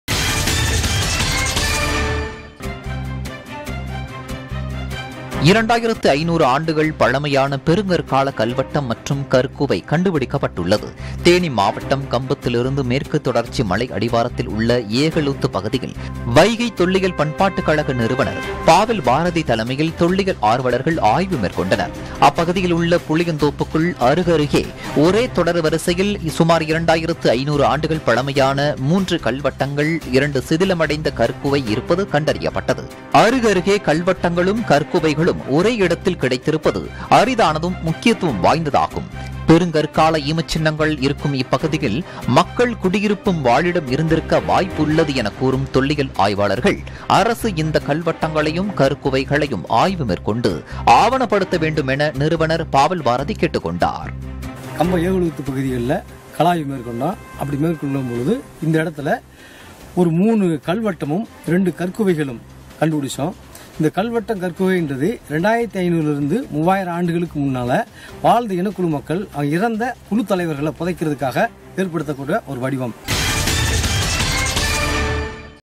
2,500 ஆண்டுகள் பழமையான பெருங்கற்கால கல்வட்டம் கண்டுபிடிப்பு | செய்தித் துளிகள் | PuthuyugamTV
தேனி -  2,500 ஆண்டுகள் பழமையான பெருங்கற்கால கல்வட்டம் கண்டுபிடிப்பு  | செய்தித் துளிகள் | PuthuyugamTV  |